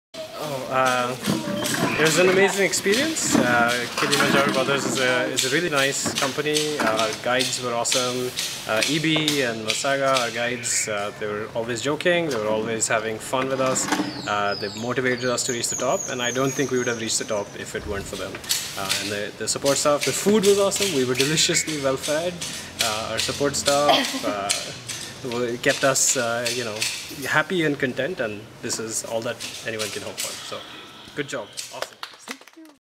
PROFESSOR DHRUV FROM USA CLIMBED KILIMANJARO WITH KILI BROTHERS
It was a great Pleasure to work with  Professor Dhruv from New York city - USA